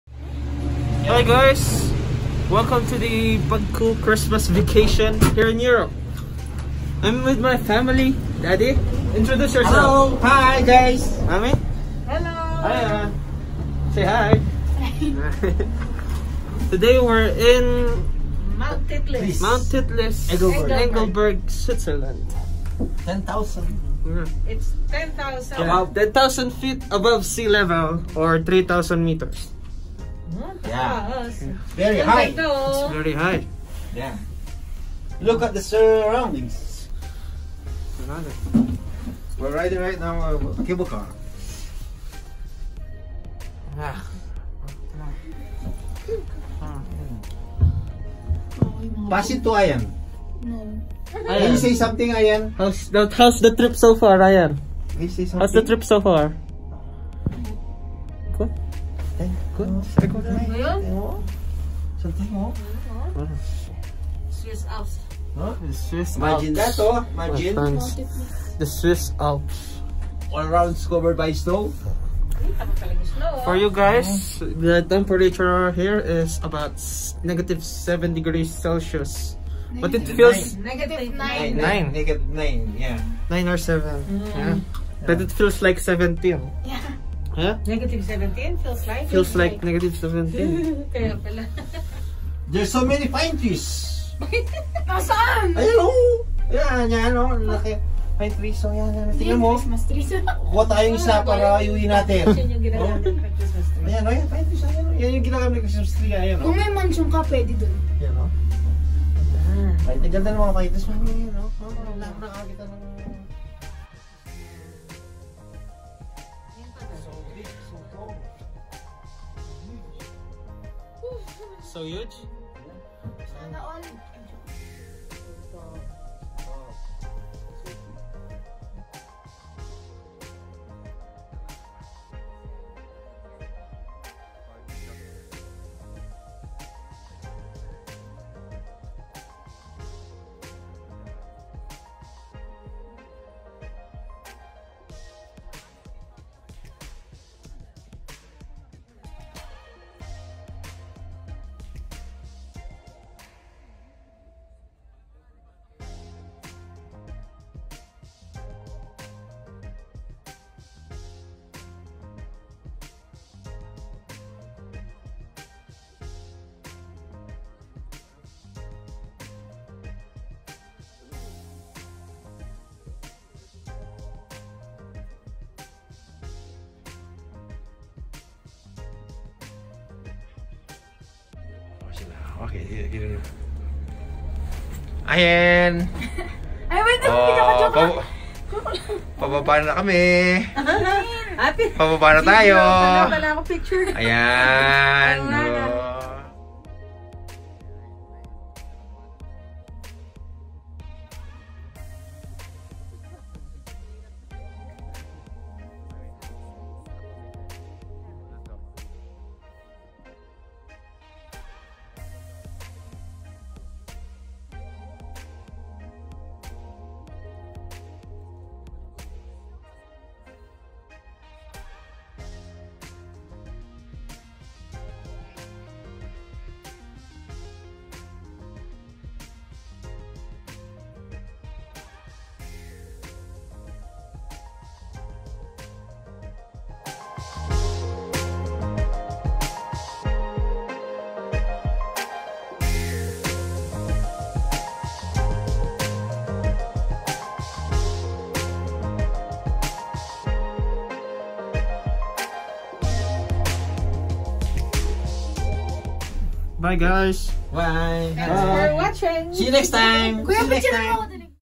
Riding a Cable Car in Mt.Titlis, Switzerland | VLOG #1
Vlogging our Christmas trip in Switzerland (December 2019)